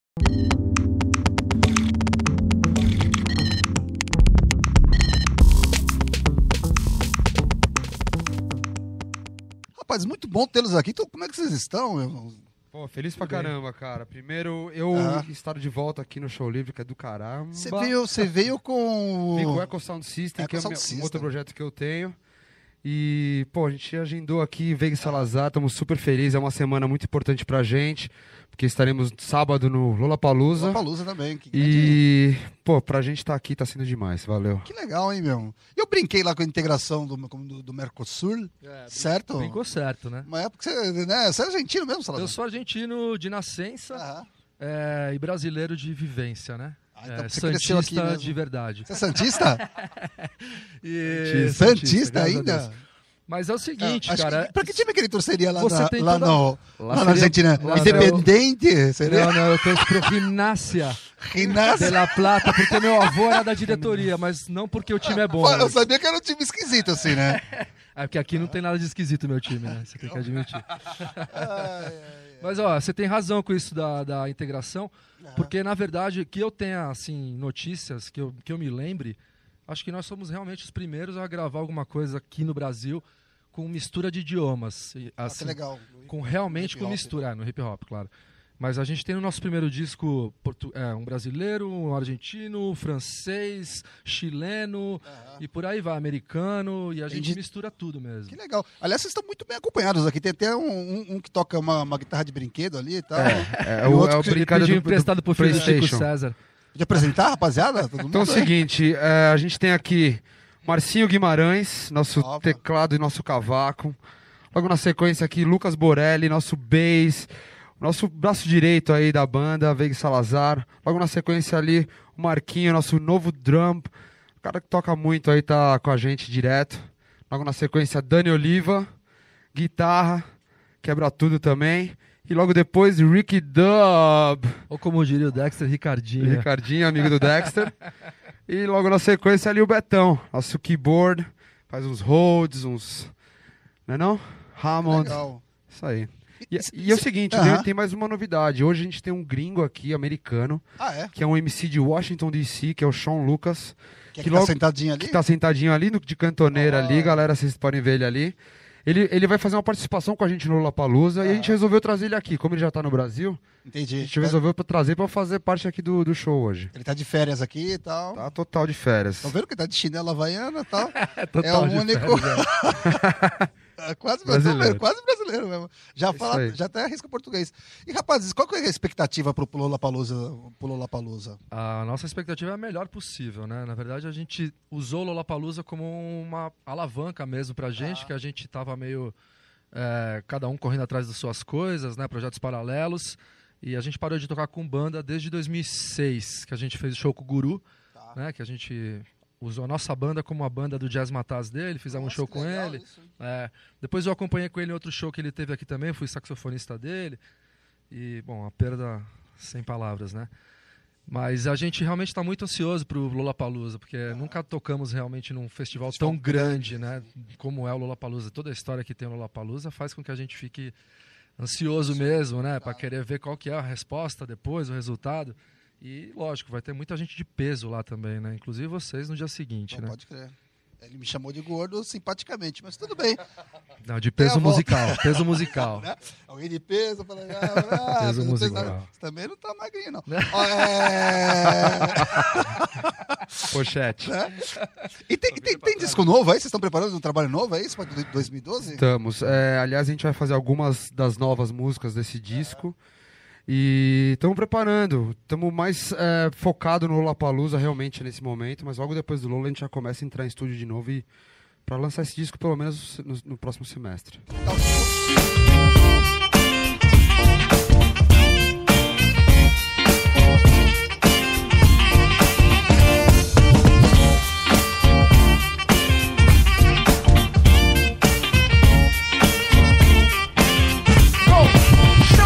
Veiga & Salazar no Estúdio Showlivre 2012 - Entrevista
A dupla Veiga e Salazar é o destaque do Estúdio Showlivre transmitido ao vivo no dia 03 de abril de 2012.

Confira a playlist com os clipes em HD do Veiga e Salazar no Showlivre

Perfil:
Veiga é Gustavo: brasileiro, MC e multi-instrumentista. Salazar é Andrés: argentino, saxofonista e MC. Suas músicas contém uma mistura de jazz, bossa nova, soul, raggae e hip hop.

Depois de um hiato a dupla voltou com tudo esse ano, comemorando 10 anos de carreia em grande estilo  no Lollapalooza do dia 07/04. Aqui no Estúdio Showlivre apresentaram um pouco do trabalho novo que está em fase de gravação e previsto para o segundo semestre desse ano.